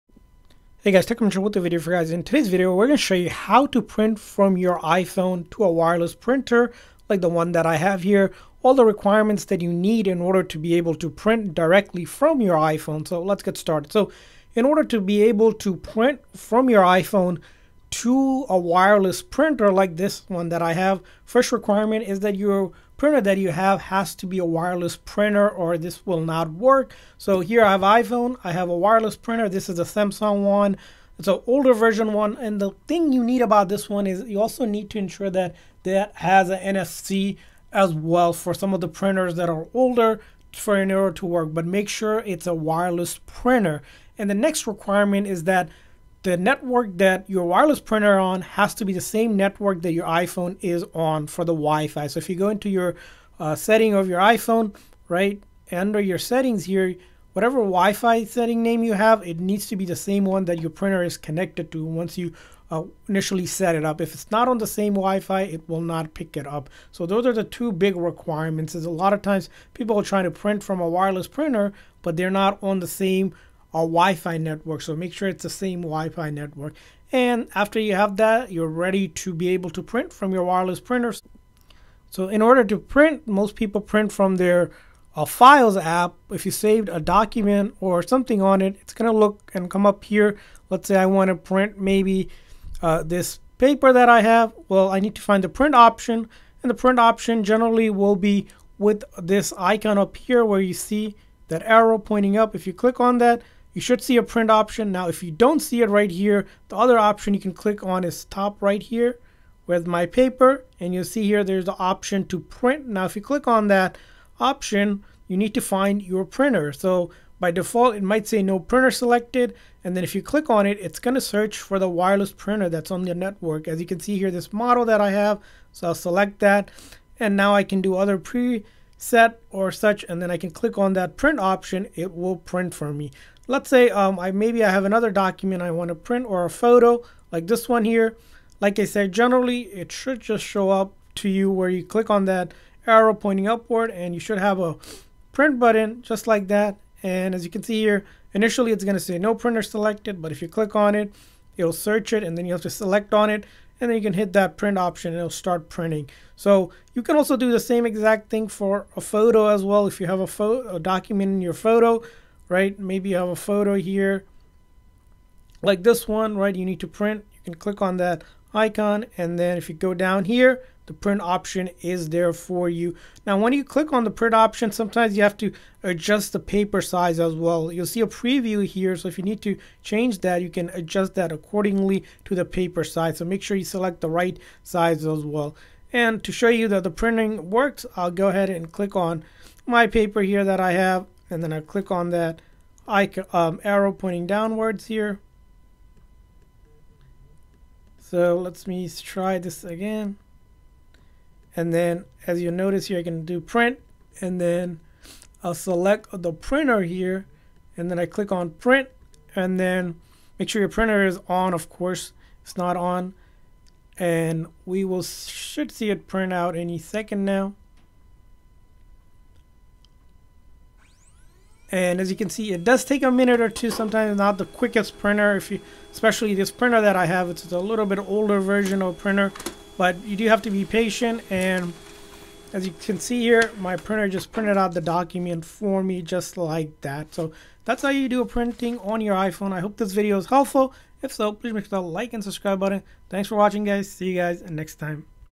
How to Print from iPhone to Wireless Printer
In this tutorial we teach you how to print from your iPhone to a wireless printer on your network. First we discuss the requirements to be able to print from iPhone. Next we show you the options available to print on your iPhone. Finally, we demo a paper we print using our iPhone from the Files app.